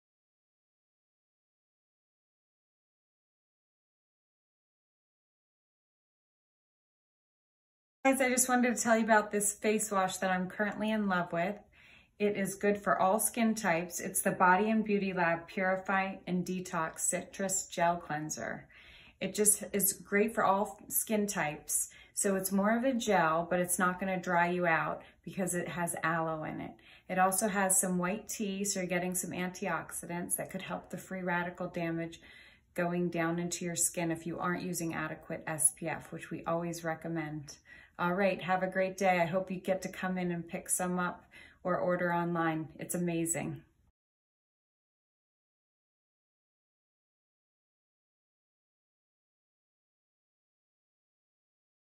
Citrus Gel Cleanser
Medical Injector Sarah Sidiqi MSN, FNP reviews the Body+Beauty Lab Citrus Gel Cleanser